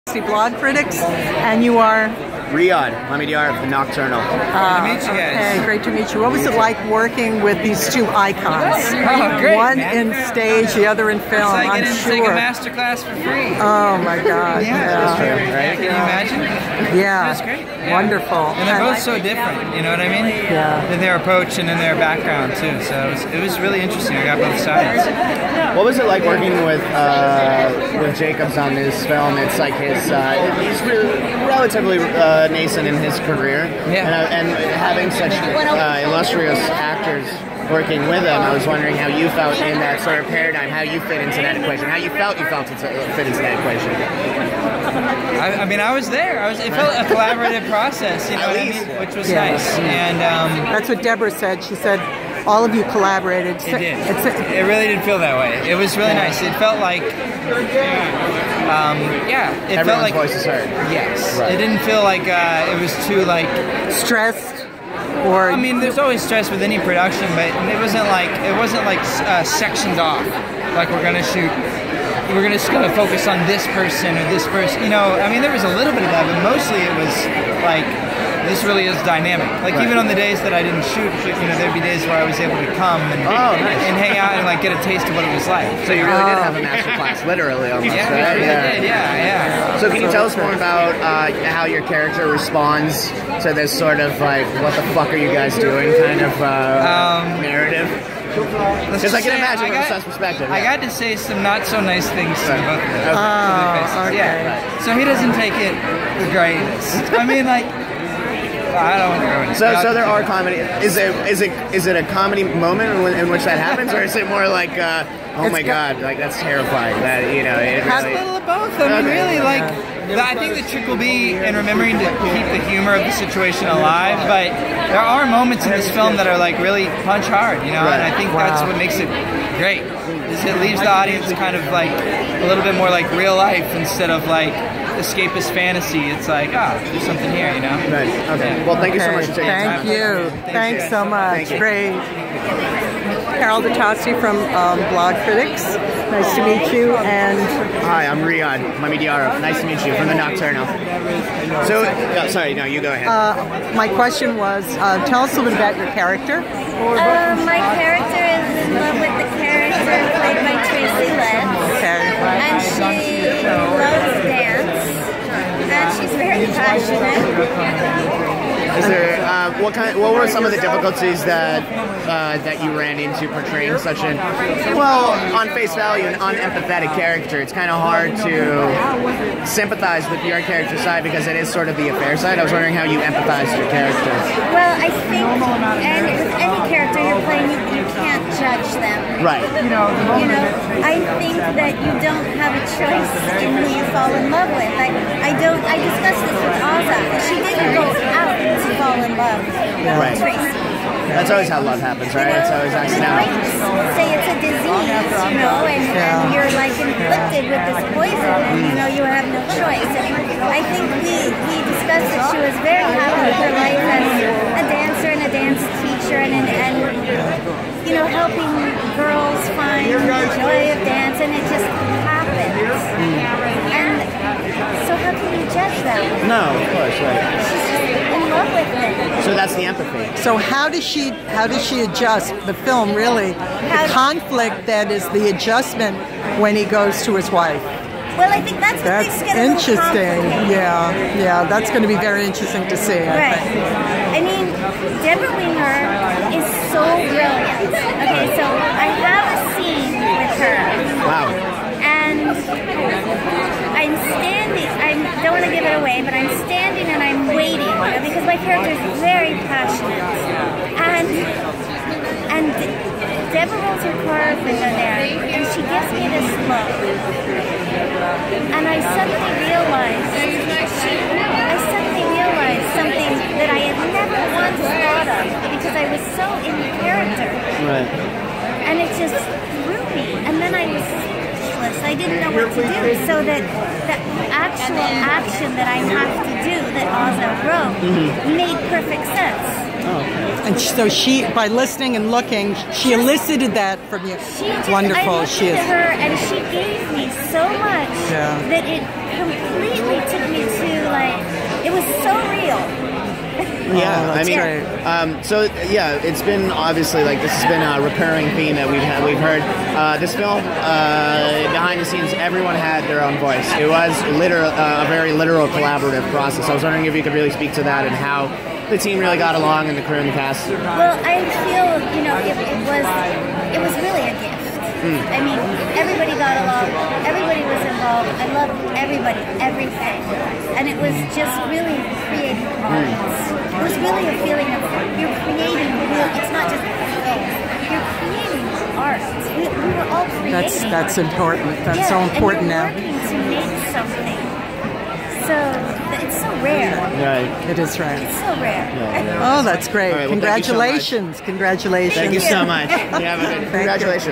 Tyler Ross and Melora Walters Talk 'The Lovers' at Tribeca FF
Tyler Ross and Melora Walters discuss their character and working with acting icon Debra Winger and multi-talented Tracy Letts. Both play the married couple turned lovers and cheaters on their extra-marital partners. 'The Lovers' is the complicated, human comedy of love, marriage, cheating, relationship faithfulness and unfaithfulness in a humorous mash-up which reveals the poignancy, trials, tribulations and love in a long-term marriage.